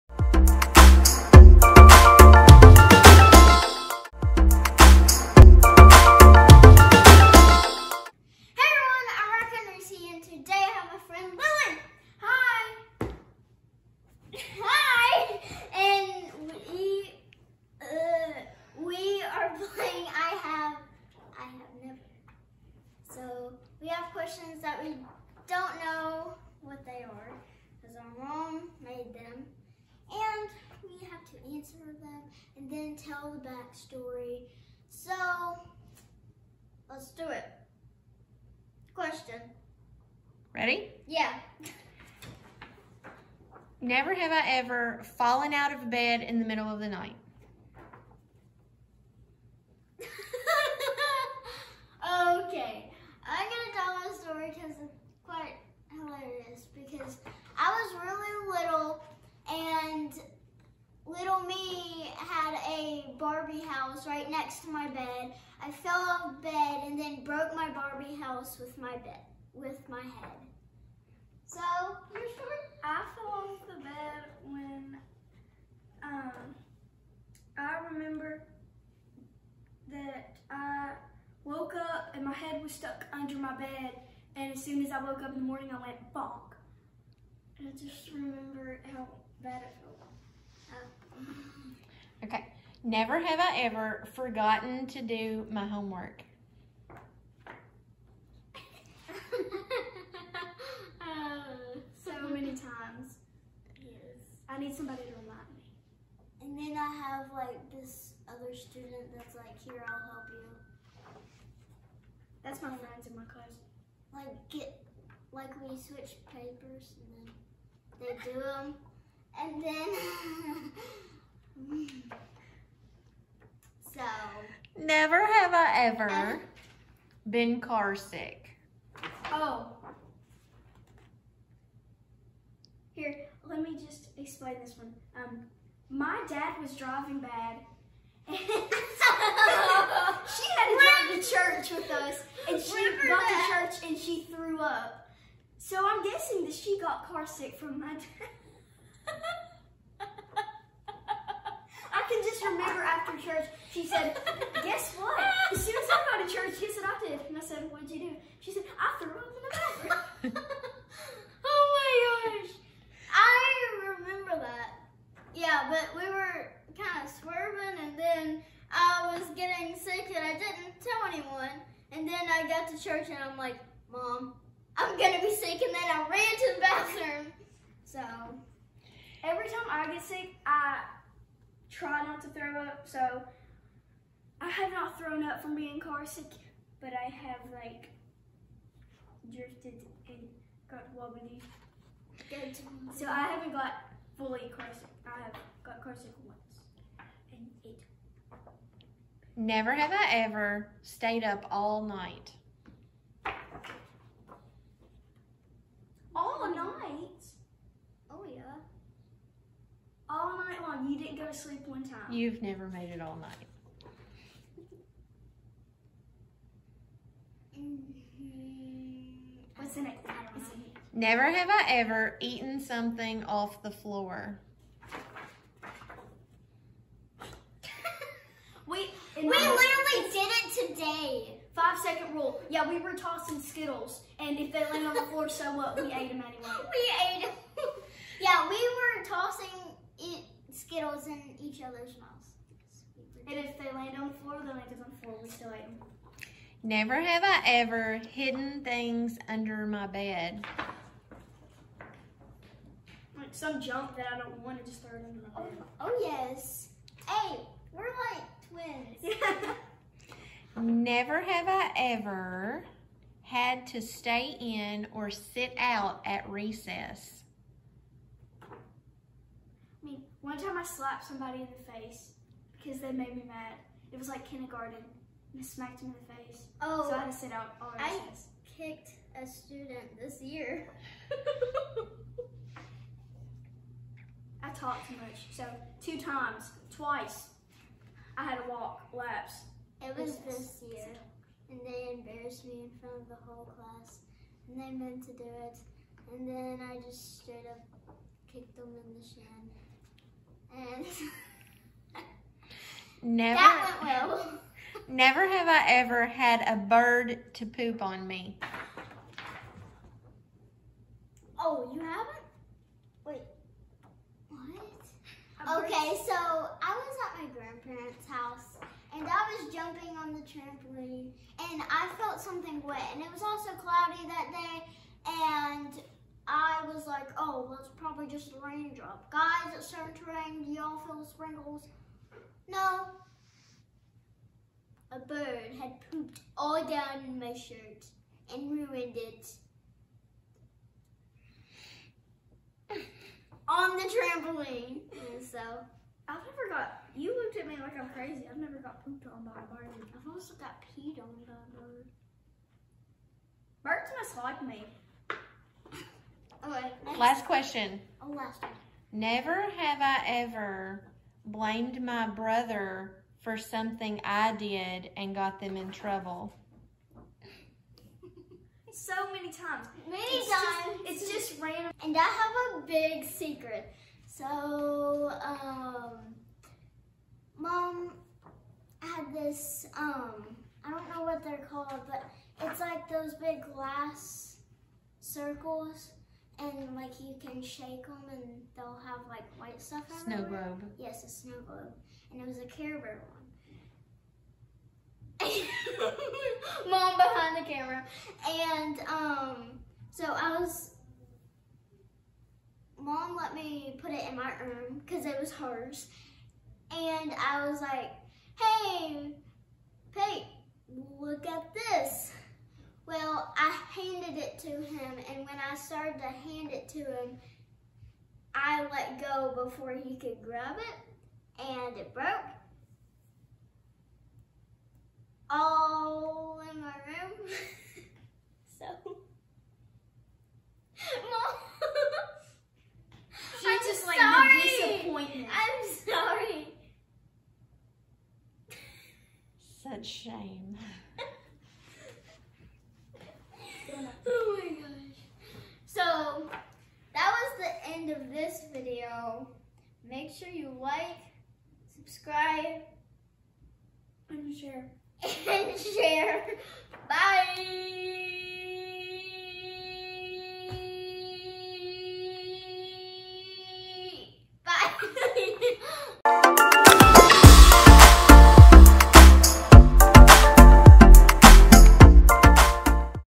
I Have, I Have Never with Lily | RockinReesey
RockinReesey plays neverhaveiever with her BFF, Lily!